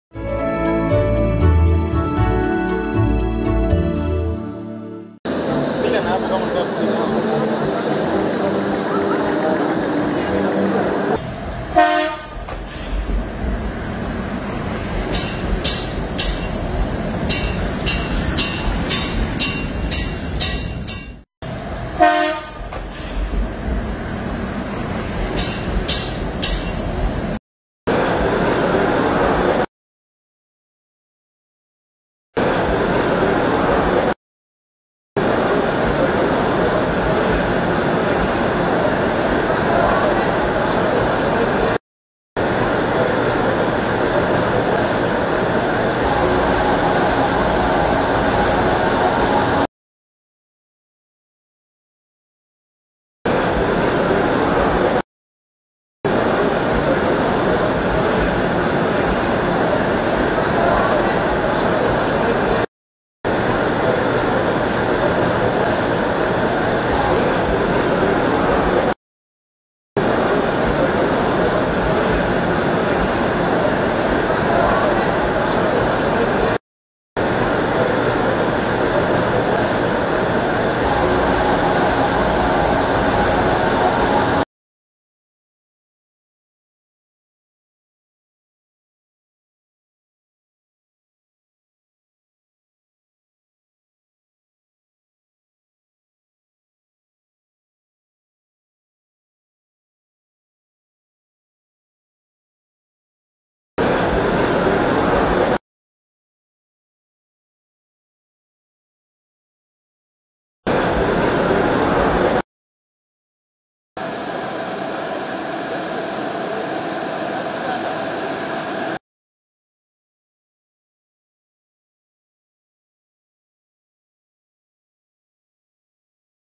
Inside the Nairobi Commuter Rail DMU train, Uhuru ride along
A sneak peek inside the interior of the newly launched Nairobi Commuter Rail DMU trains, President Uhuru rides on the DMU train
This is a new mode of transport in Kenya as many kenyans have been using matatus to commute